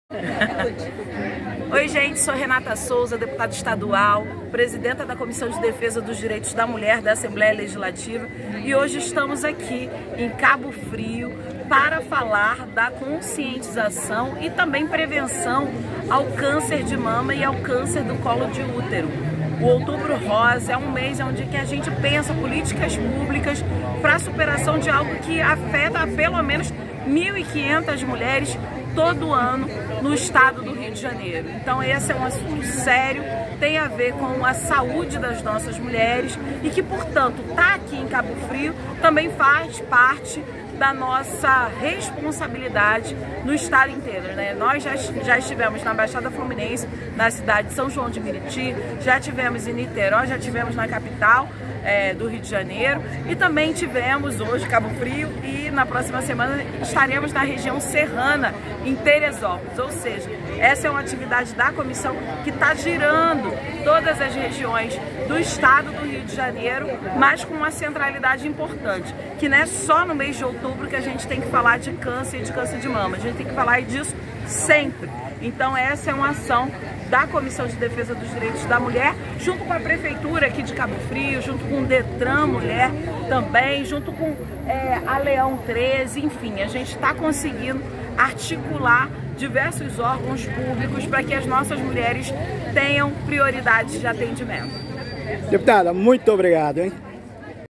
DEPUTADA RENATA SOUZA (PSOL) PARTICIPA DA CAMPANHA OUTUBRO ROSA EM CABO FRIO
O Eterno Aprendiz pirou e entrou no clima do sextou, fechou as portas dando folgo aos componentes da redação; mas como comunicação é uma cachaça e vicia ao chegar na Praça Porto Rocha, Centro de Cabo Frio deparou com a campanha do Outubro Rosa, de prevenção ao câncer de mama, em Cabo Frio, com a presença da deputada Renata Souza (PSOL) participando de uma ação de conscientização e informações sobre o câncer de mama, apoio jurídico e alertas sobre violência contra a mulher, o evento também ofereceu serviços da Fundação Leão XIII, do Detran Mulher, entre outros.

A parlamentar, que é presidenta da Comissão de Defesa dos Direitos da Mulher da Alerj, contou que a comitiva já percorreu diversos pontos do estado como na capital, Niterói, São João de Meriti e vai nos próximos dia para o municipio de  Teresópolis, Região Serrana levando serviços voltados à mulher em parceria com as prefeituras.